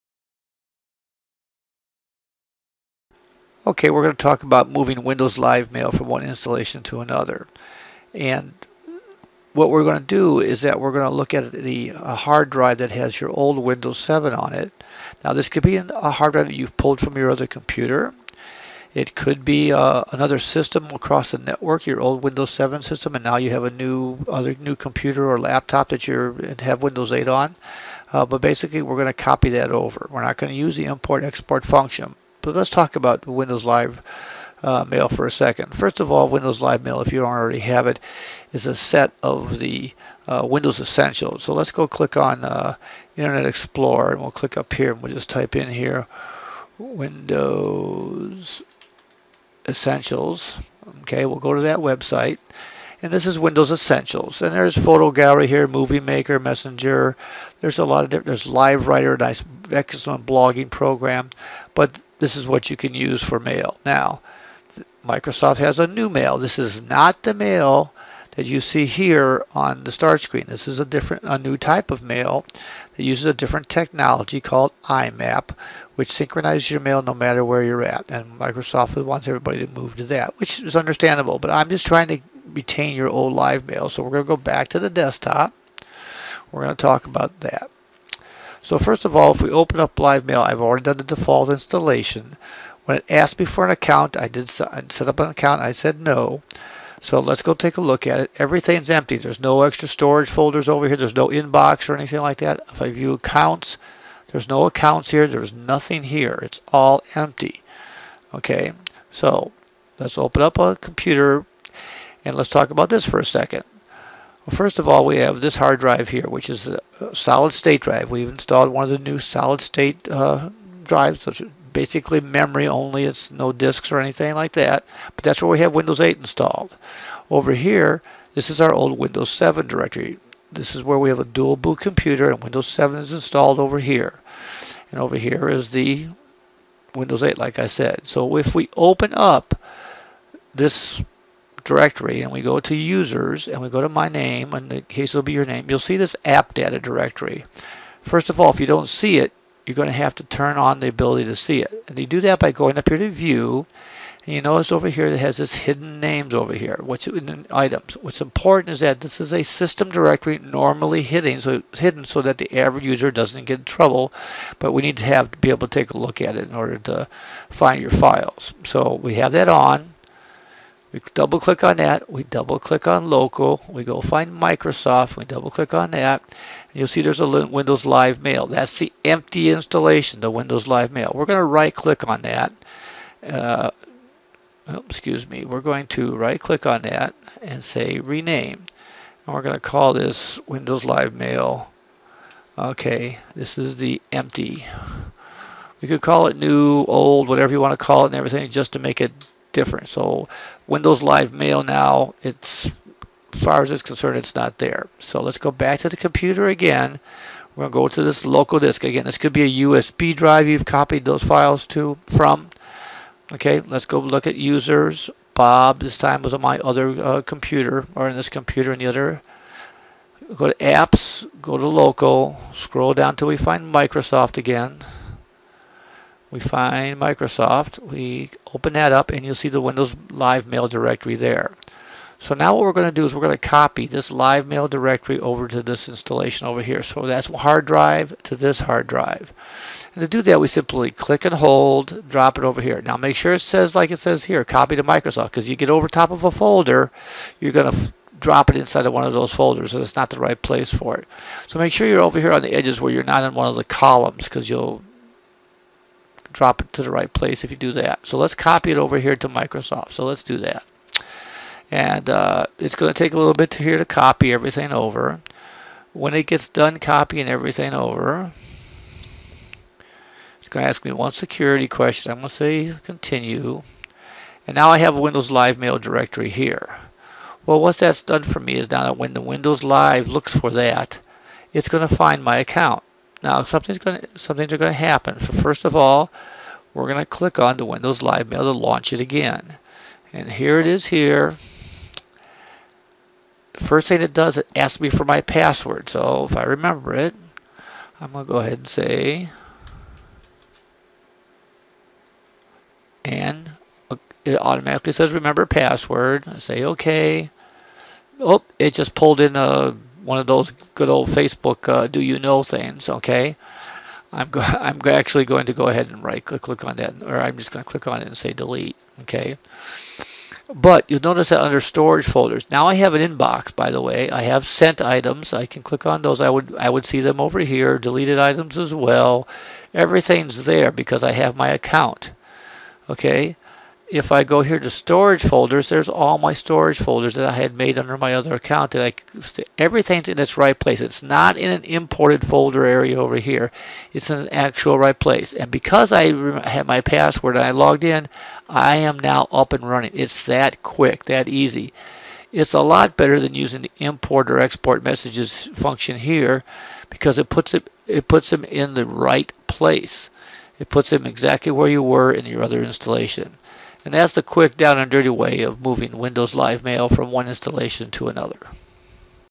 Windows 8 Tips: Moving Live Mail From Windows 7
This is the unoffical way of moving your old Live Mail to your new Windows 8 installation.  We are assuming you have a new system or choose to install Windows 8 to dual boot with Windows 7.  Therefore you still can access your old Windows 7 files.